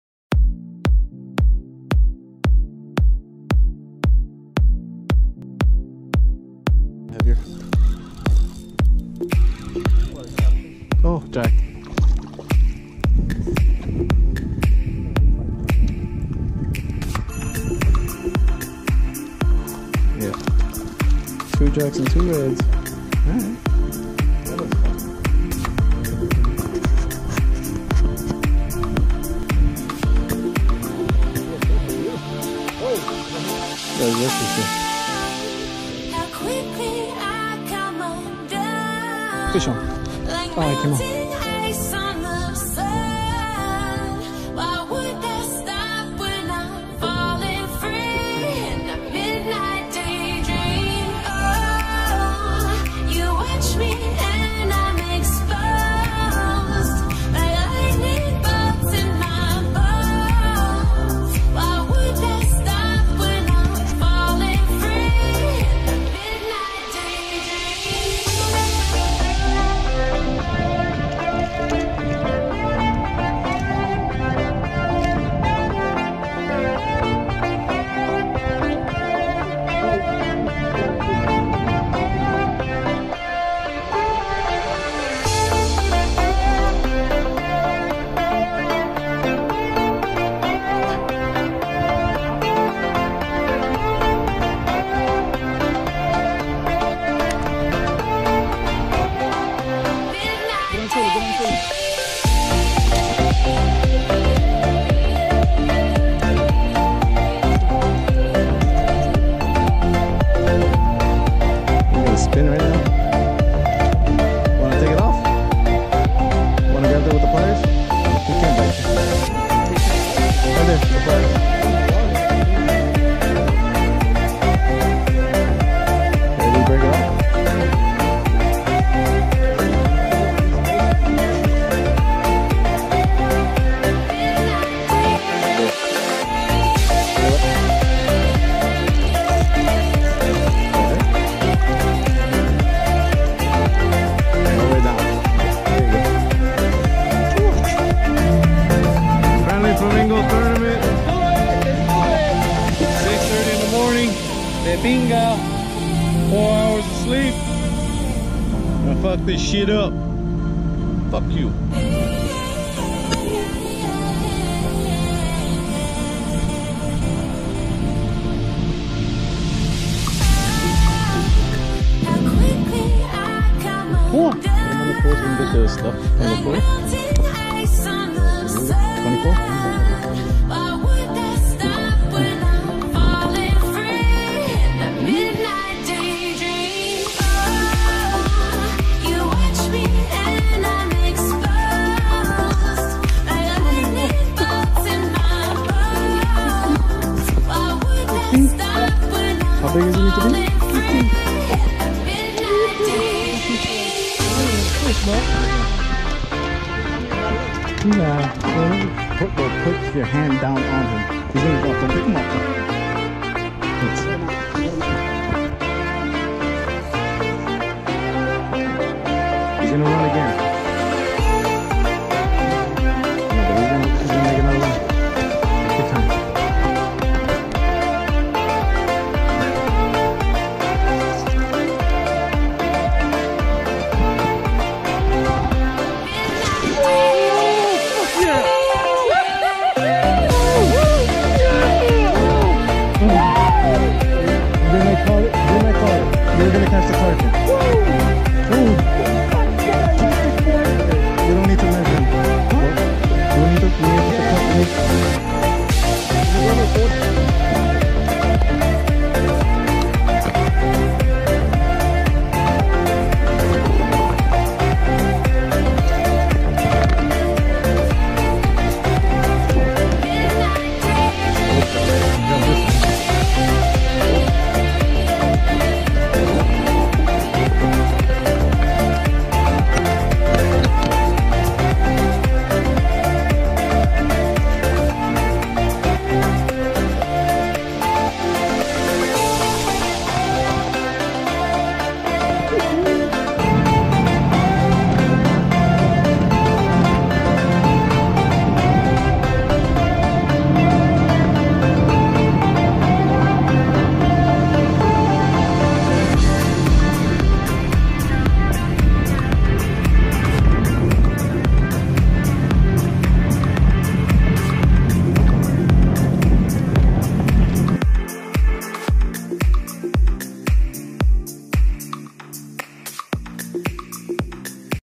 Flamingo Fishing Tournament
My buddy and I pre fishing and fishing the FFT series fishing tournament. We caught 3 out of 4 species and had a lot of fun on my Salt Marsh 1656V skiff.